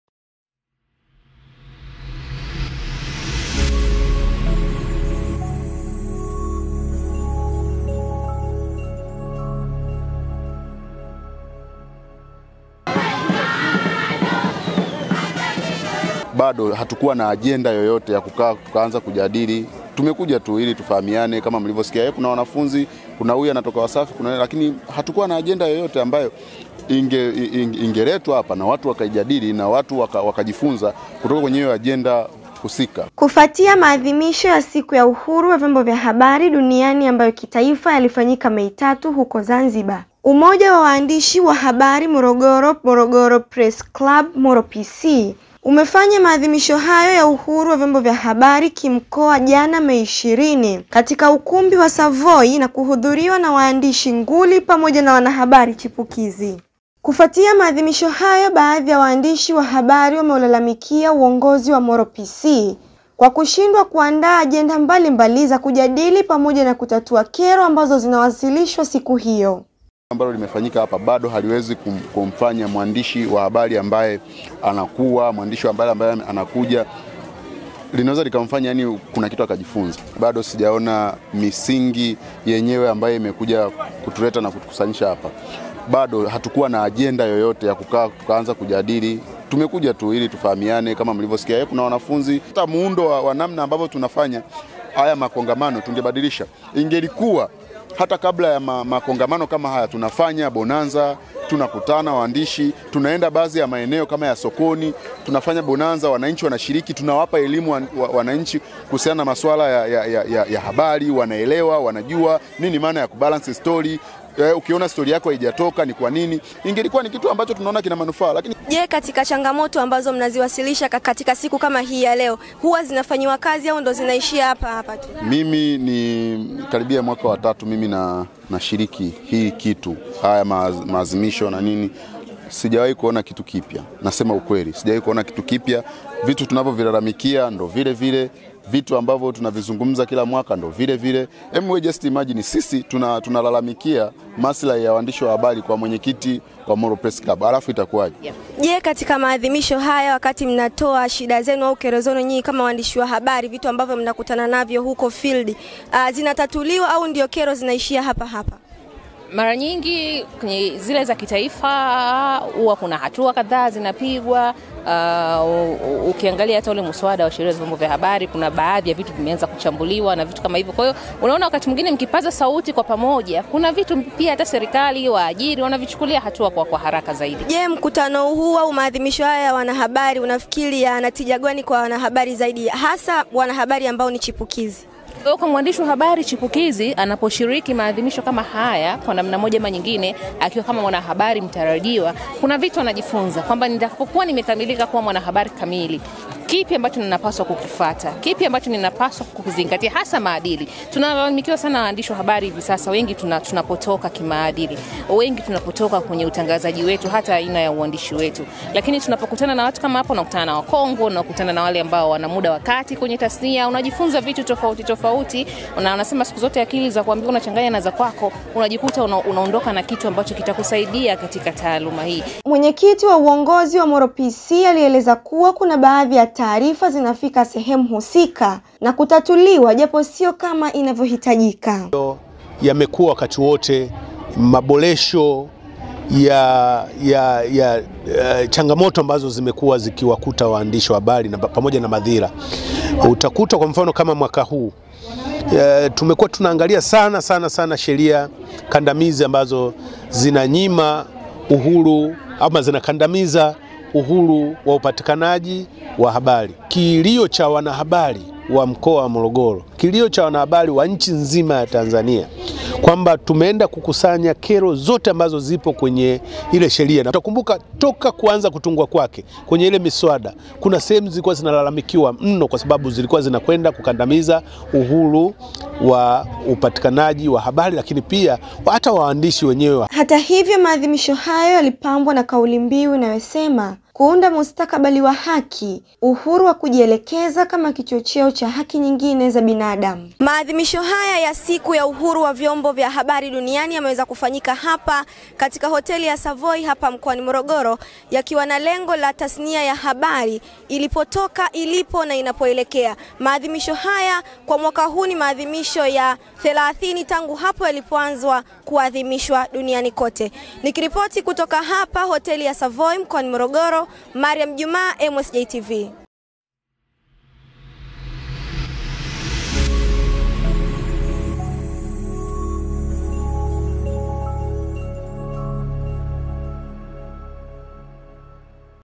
MAADHIMISHO YA UHURU WA VYOMBO VYA HABARI MOROGORO #2023 MAKUBWA YAJADILIWA/VIONGOZI WATOA TAMKO
Wanahabari kutokea mkoa wa Morogoro wakisherehesha aiku adhimu ya uhuruwavyombovyahabari 2023..

Nasi Chuo cha Uandishi wa Habari na mawaailiano kwa Umma (MOROGORO SCHOOL OF JOURNALISM), Wadau muhimu katika tasnia hii ya habari tumeungana na wanahabari wenzetu duniani katika kusherehekea siku hii muhimu ambayo tunaitumia kupaza sauri zetu katika mambo kadha wa kaza yanayohusu tasnia yetu.

Hii ni ripoti maalum iliyofamywa na mwanafunzi mariamJumaa @___mamuu_og